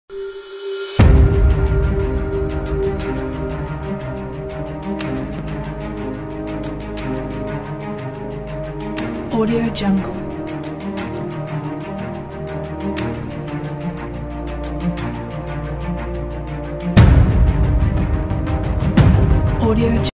After Effects , Premiere Pro , Final Cut , Edius Template hexagonal Particle futuristic background
Hexagonal Particle Background

You can use this in your documentaries, films, tv shows. capable with : After effects, Adobe Premiere , Edius , or another softwares. used music